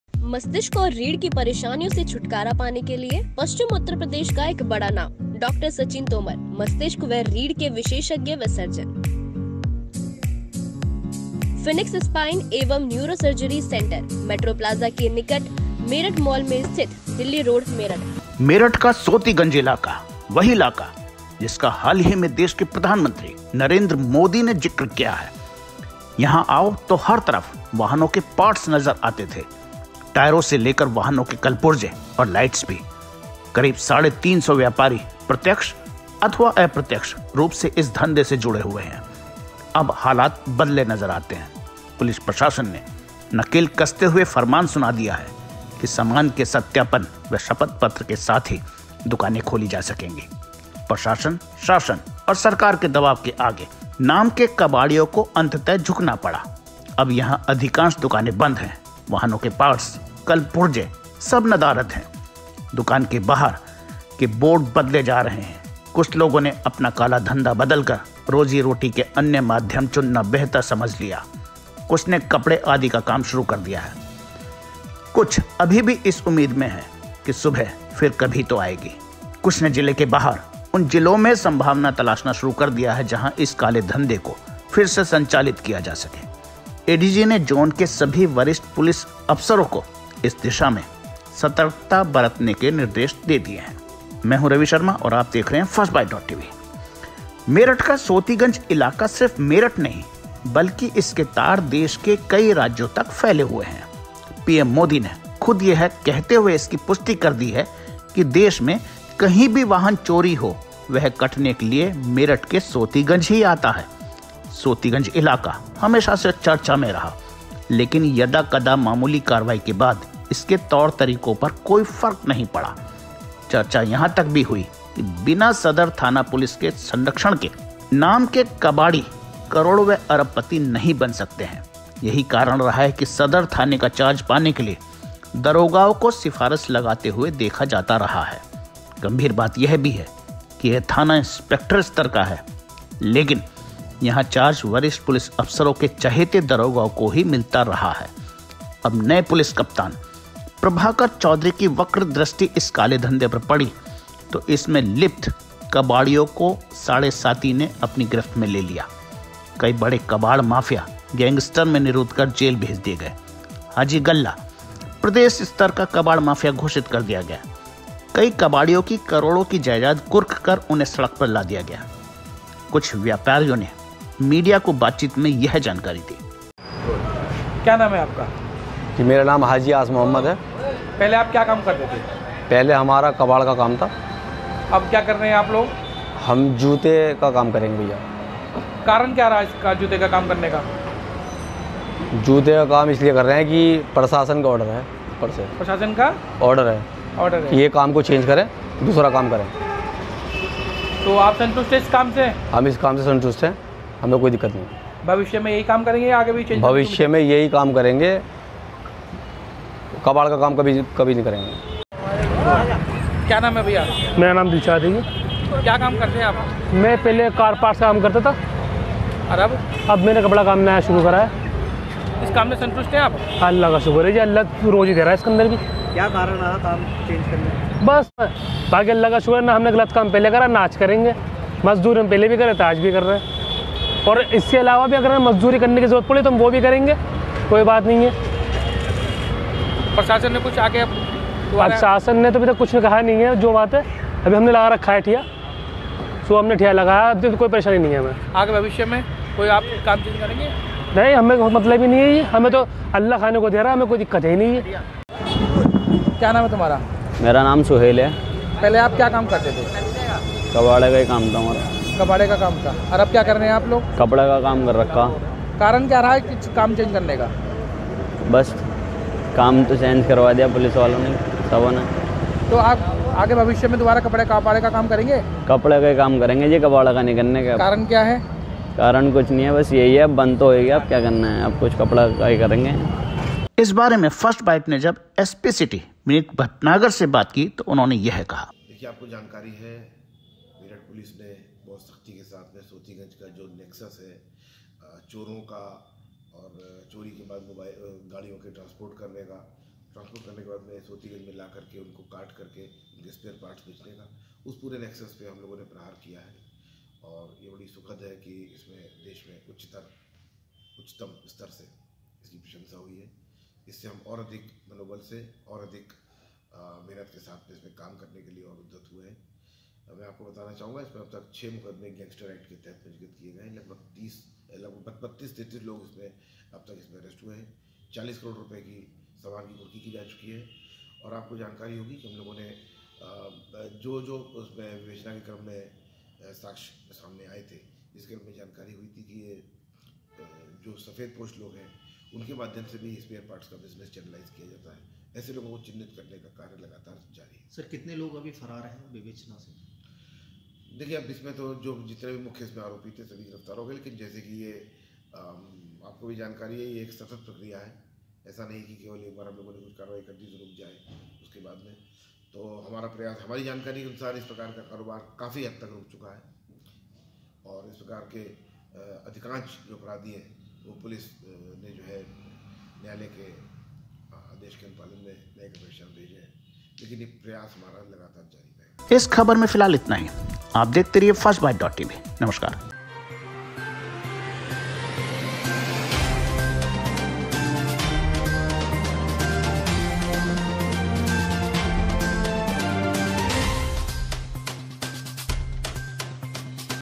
वाहनों के पार्ट्स नहीं, अब बिकते हैं कपड़े व जूते, कबाड़ियों ने काम बदला । Firstbytetv
मेरठ का सोतीगंज इलाका...वही इलाका जिसका हाल ही में देश के प्रधानमंत्री नरेंद्र मोदी ने जिक्र किया है। यहां आओ तो हर तरफ वाहनों के पार्ट्स नजर आते थे, टायर से लेकर वाहनों के कलपुर्जे और लाइट्स भी। करीब साढ़े तीन सौ व्यापारी प्रत्यक्ष अथवा अप्रत्यक्ष रूप से इस धंधे से जुड़े हैं। अब हालात बदले नजर आते हैं, पुलिस प्रशासन ने नकेल कसते हुए फरमान सुना दिया कि सामान के सत्यापन व शपथपत्र के बाद ही दुकान खोली जा सकेगी। प्रशासन, शासन और सरकार के दबाव के आगे नाम के कबाड़ियों को अंतत झुकना पड़ा। अब यहां अधिकांश दुकाने बंद हैं, वाहनों के पार्ट्स , कल पुर्जे सब नदारद हैं। दुकान के बाहर के बोर्ड बदले जा रहे हैं। कुछ लोगों ने अपना काला धंधा बदलकर रोजी रोटी के अन्य माध्यम चुनना बेहतर समझ लिया। कुछ ने कपड़े आदि का काम शुरू कर दिया। कुछ भी भी इस उम्मीद में हैं कि सुबह फिर कभी तो आयेगी..। कुछ ने जिले से बाहर उन जिलों में संभावना तलाशना शुरू कर दिया है जहां इस काले धंधे को फिर से संचालित किया जा सके। एडीजी ने जोन के सभी वरिष्ठ अफसरों को इस दिशा में सतर्कता बरतने के निर्देश दे दिये हैं।
मेरठ का सोतीगंज इलाका सिर्फ मेरठ नहीं बल्कि इसके तार देश के कई राज्यों तक फैले हुए हैं । पीएम मोदी ने खुद यह कहते हुए इसकी पुष्टि कर दी कि देश में कही भी वाहन चोरी हो..वह कटने के लिये मेरठ के सोतीगंज ही आता है । सोतीगंज इलाका हमेशा से चर्चा में रहा लेकिन यदाकदा मामूली कार्यवाही के बाद इसके तौर तरीकों पर कोई फर्क नहीं पड़ा । चर्चा यहां तक हुई कि बिना सदर थाना पुलिस के संरक्षण के नाम के कबाड़ी करोड़ व अरब पति नहीं बन सकते हैं। यहीं कारण रहा है कि सदर थाने का चार्ज पाने के लिये दरोगाओं को सिफारिश लगाते देखा जाता रहा है। गंभीर बात यह भी है कि यह थाना इंस्पेक्टर स्तर का है लेकिन यहां चार्ज वरिष्ठ पुलिस अधिकारियों के चहेते दरोगाओं को ही मिलता रहा है । अब नये पुलिस कप्तान प्रभाकर चौधरी की वक्र दृष्टि इस काले धंधे पर पड़ी तो इसमें लिप्त कबाड़ियों को साढ़े साती ने अपनी गिरफ्त में ले लिया । कई बड़े कबाड़ माफिया गैंगस्टर में निरुद्ध कर जेल भेज दिये गये। हाजी गल्ला प्रदेश स्तर का कबाड़ माफिया घोषित कर दिया गया । कई कबाड़ियों की करोड़ों की जायदाद कुर्क कर उन्हें सड़क पर ला दिया गया ।।